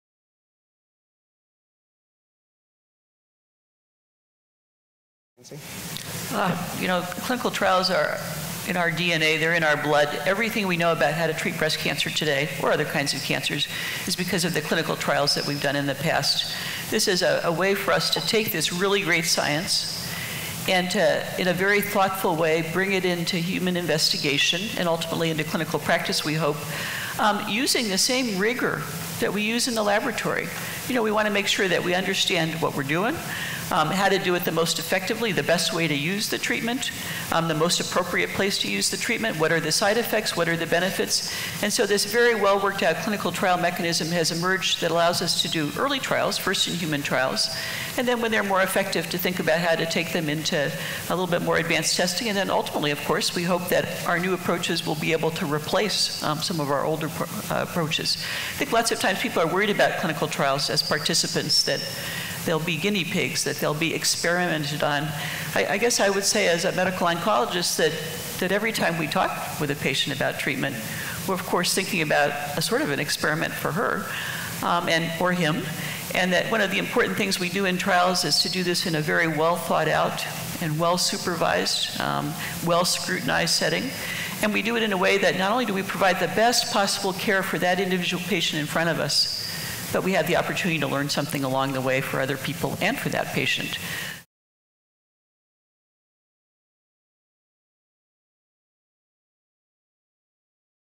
Why Are Clinical Trials So Important in Cancer Research?
There are many misconceptions about clinical trials. Watch Dr. Nancy Davidson of the University of Pittsburgh Cancer Institute as she explains why clinical trials are misunderstood — and why they're so important for the future of cancer research during The Breast Cancer Research Foundation's Oct. 15, 2013, Symposium and Luncheon in New York City.